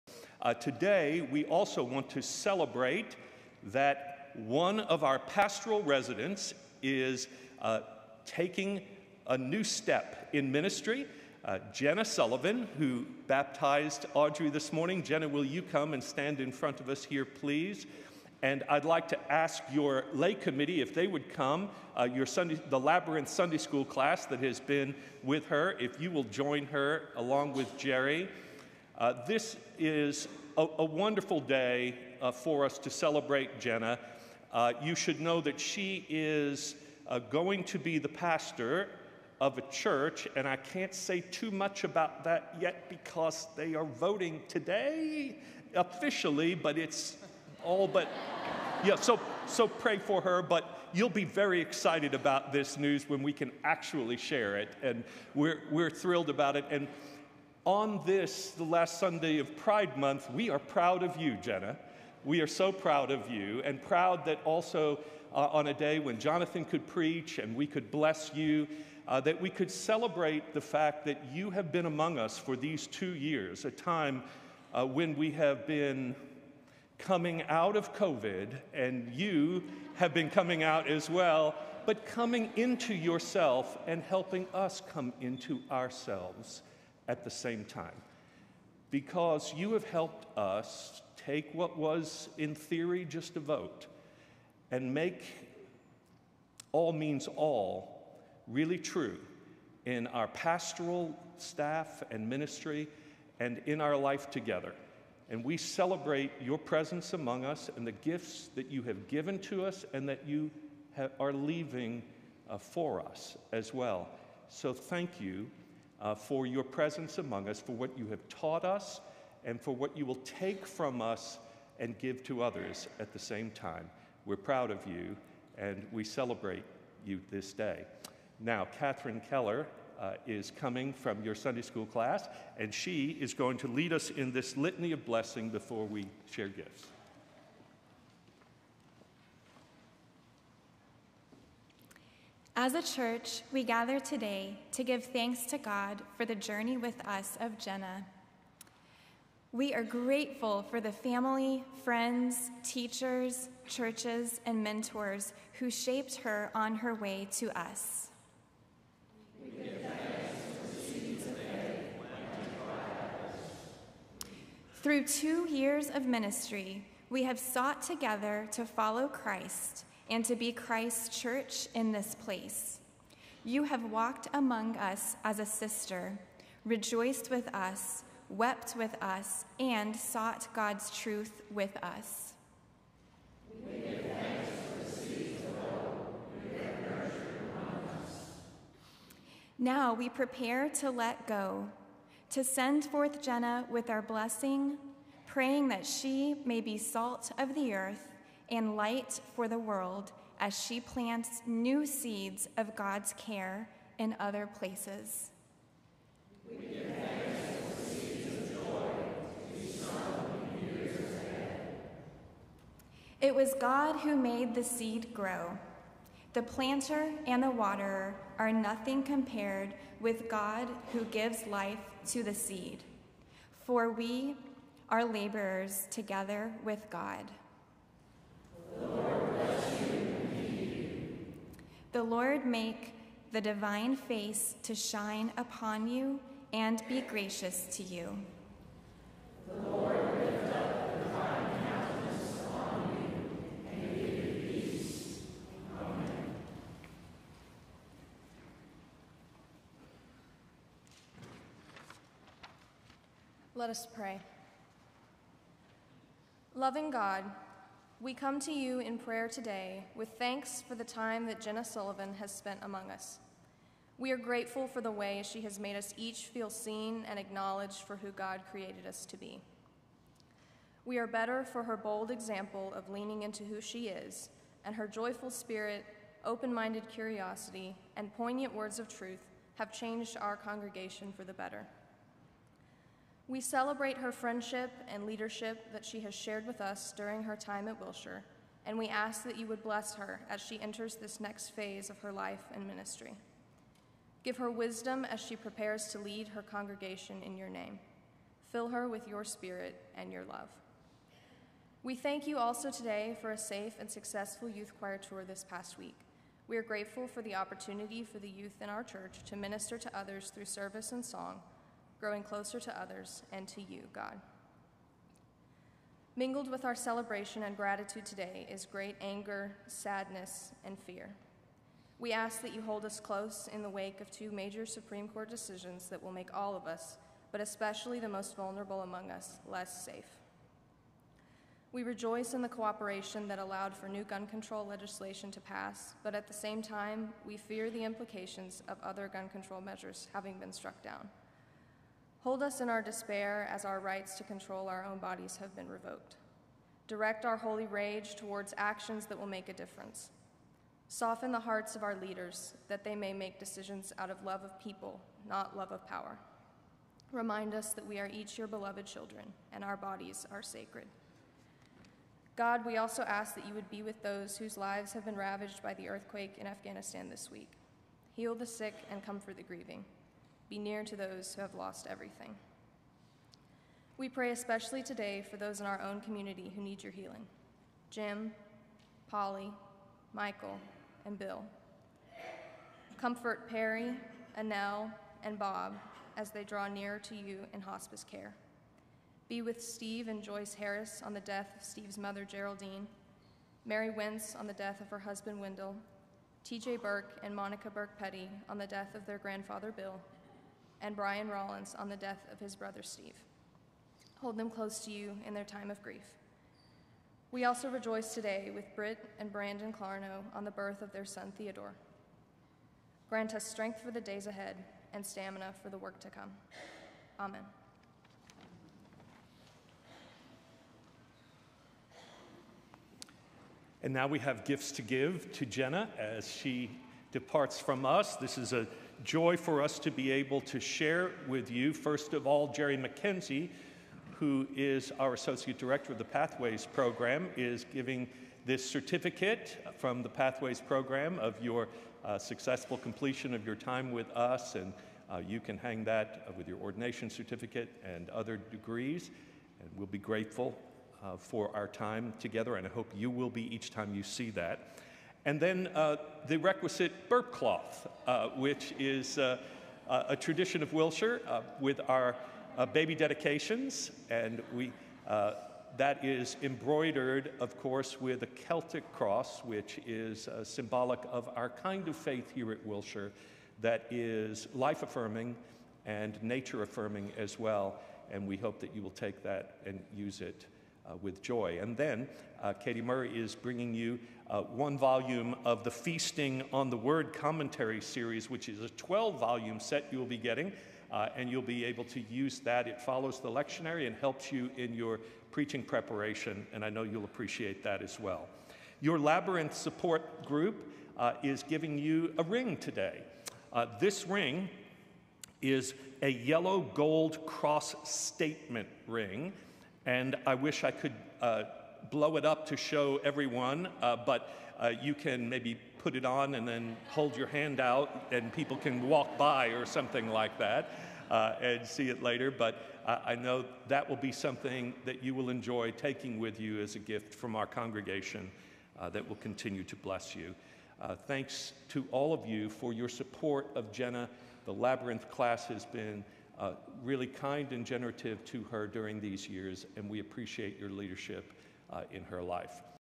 Blessing service for Pastoral Resident Jenna Sullivan
Wilshire’s worship service on June 26, 2022, included a time of recognition and blessing for Pastoral Resident Jenna Sullivan. Jenna is nearing the end of her two years in Wilshire’s pastoral residency program.

Connect with Wilshire by filling out a guest card, making a contribution or submitting a prayer request

The Wilshire mission is to build a community of faith shaped by the Spirit of Jesus Christ. Partnered with the Cooperative Baptist Fellowship.